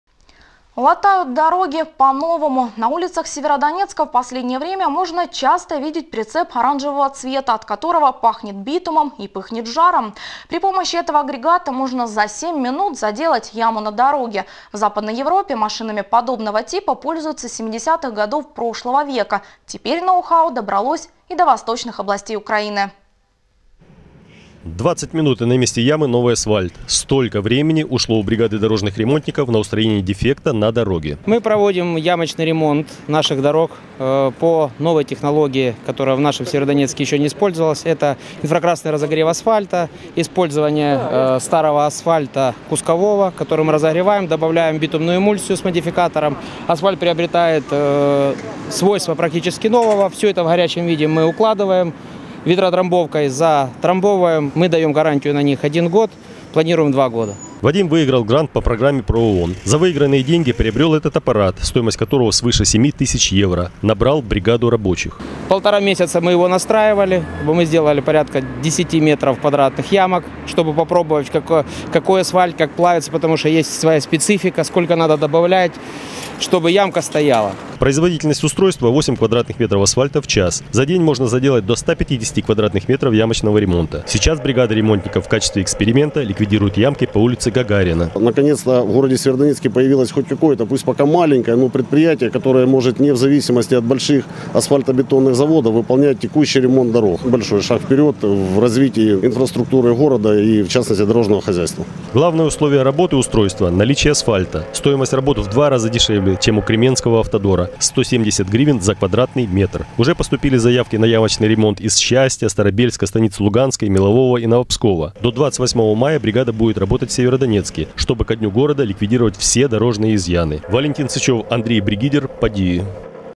Техника для ремонта дорог | отзыв о TM TICAB | Feedback for TICAB
Телеканал UA:ДОНБАС рассказывает об использовании техники TM TICAB в Северодонецке
Производственно-торговая компания TICAB (Польша) представляет технику для ремонта и обслуживания дорог.